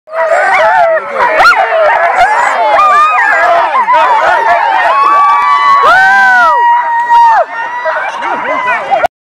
Lynn Whipple and her sled dogs at the starting line
Excitement and anticipation as Lynn Whipple and her sled dogs are at the starting line of the Grand Mesa Sled Dog Challenge 2014!